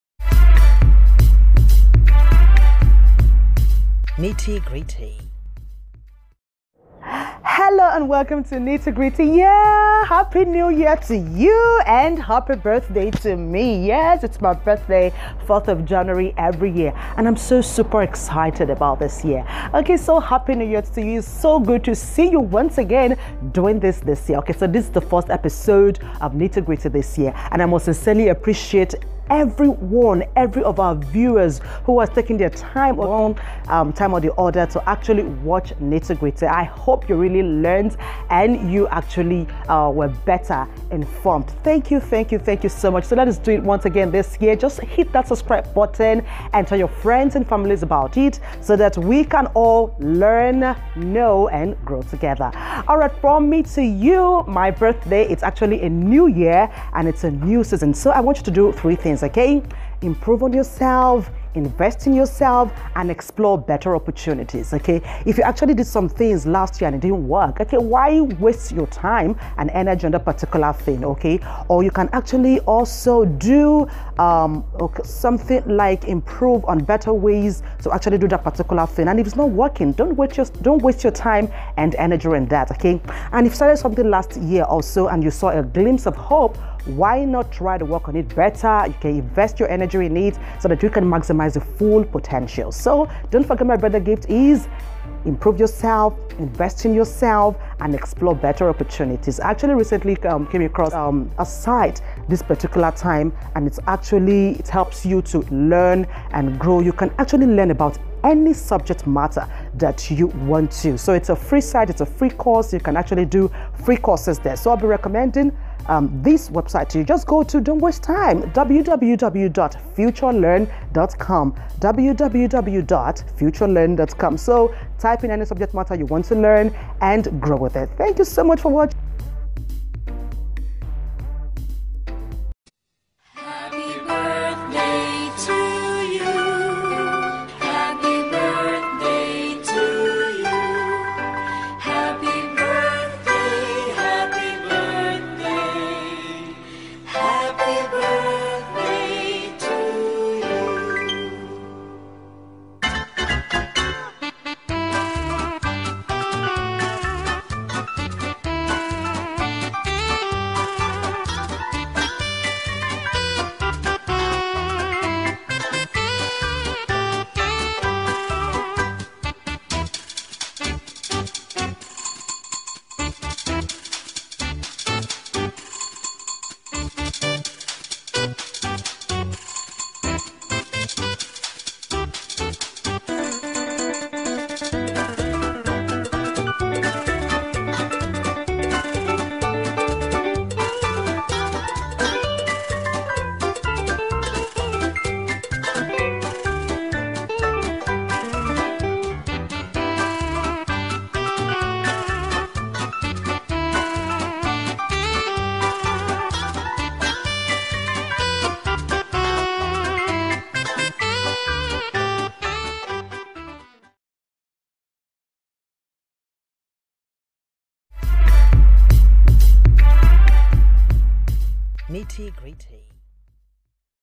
Nitty-Gritty with Nora Ep 13 (Learn how to Improve yourself)
Happy new year to you and Happy birthday to me. It's 4th of January & it's my birthday.

Learn how to Improve yourself with this video.